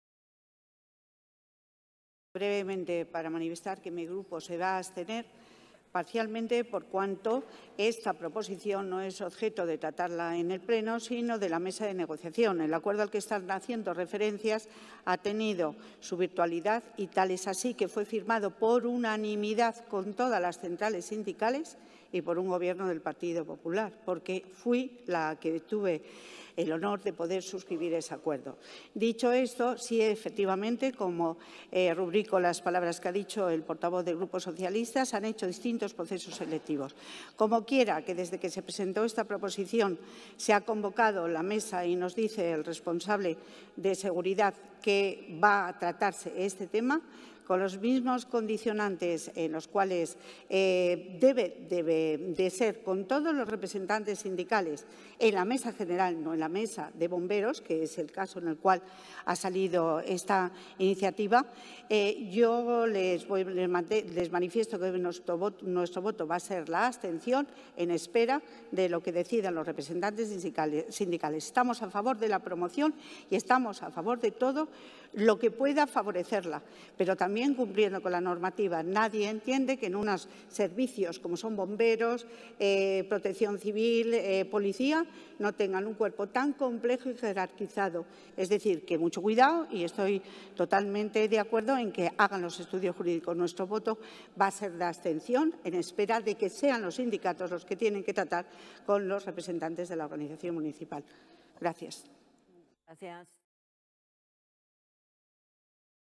Larrainzar pide al Ayuntamiento que se reuna con los sindicatos para mejor el reglamento de Bomberos
Intervención de Begoña Larrainzar en el Pleno Ordinario del Ayuntamiento de Madrid de 31 de enero de 2017. Punto 16.

Proposición interesando que se inste al Área de Gobierno de Salud, Seguridad y Emergencias a realizar las gestiones oportunas, conducentes a modificar, de conformidad con los representantes de los trabajadores, el artículo 126 apartado 4 del Acuerdo sobre mejora de la calidad de los servicios y de las condiciones de empleo de los empleados públicos en el periodo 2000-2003 (BOAM 25 de octubre de 2001), con la finalidad de permitir el acceso, en los términos previstos en el artículo 12 del Reglamento del Cuerpo de Bomberos, a la categoría de Jefe de Grupo desde la categoría de Bombero.